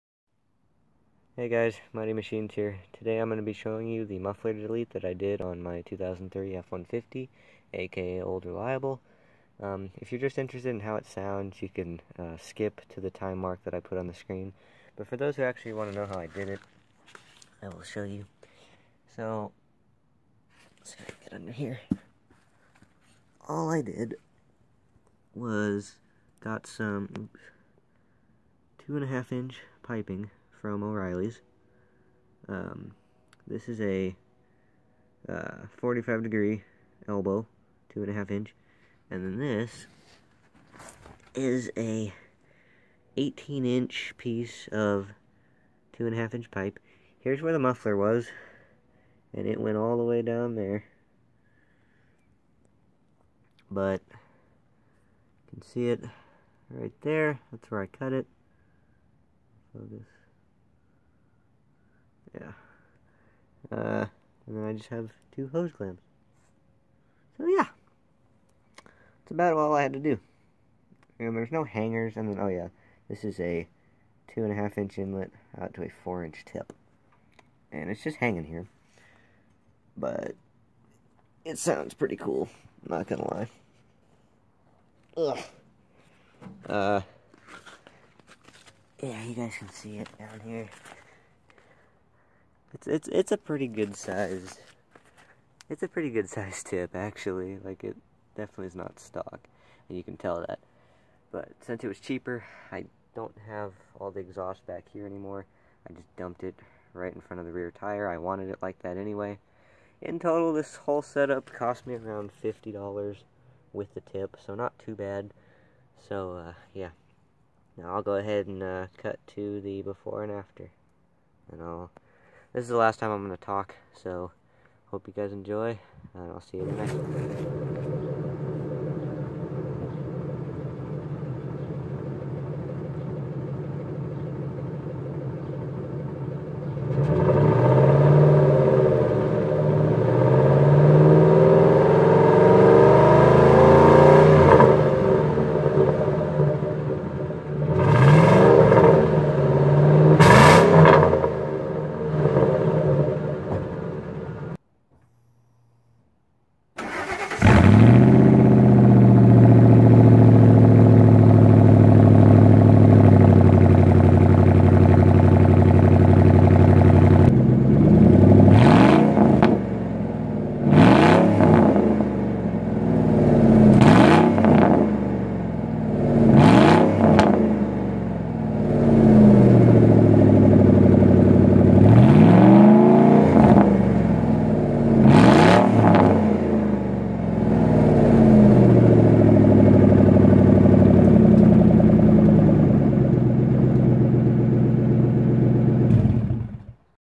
2003 Ford F-150 4.6L Muffler Delete (9/6/19)
I finally got around to muffler deleting Old Reliable! It sounds 100x better! Not too loud but not too quiet, video quality really doesn't do justice.